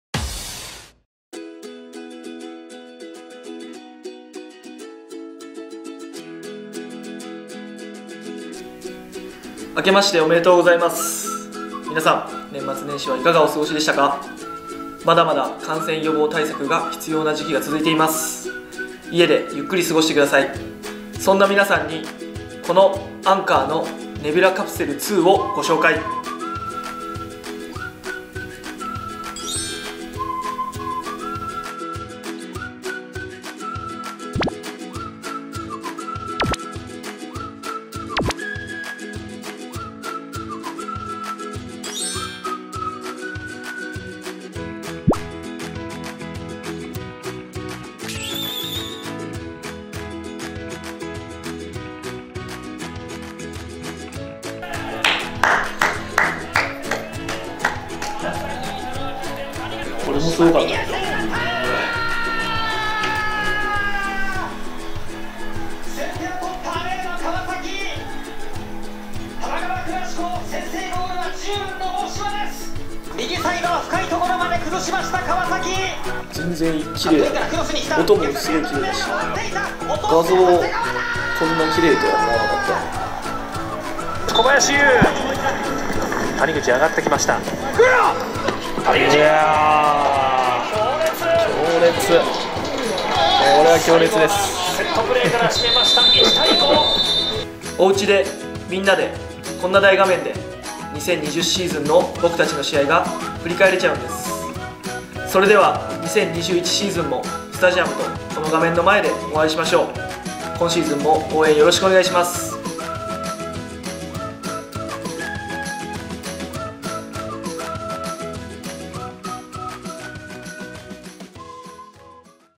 【公式】新しい生活様式の楽しみ方（谷口彰悟編）
トップチームやアカデミー選手を含めたクラブに対しての物品提供をはじめとして、製品を通じて新しいデジタルライフスタイルを日頃からご提案いただいているのが、川崎フロンターレのオフィシャルスポンサーでもあるアンカー・ジャパン株式会社様。

最近でこそ徐々に緩和されてきたとはいえ、感染予防対策および拡散防止のために、今までとは違うスタイルでの生活が続く当時、選手たちがご提供いただいたAnker製品を愛用し、厳しく自分を追い込むトレーニングに励んでいる様子や、ご家族と楽しみながら過ごしているとの情報を聞きつけ…

この状況を少しでも楽しく、充実できるよう、フロンターレのサポーターの皆さんにも喜んでいただけるような情報を！と、担当スタッフと意気投合。

ANKERとフロンターレが、新しい生活様式の楽しみ方を皆さんにお届けします!!

第5弾は、ショウゴこと谷口彰悟選手が登場です!

◆商品紹介
Nebula Capsule Ⅱ

世界初! Android TV 9.0 搭載のモバイルプロジェクター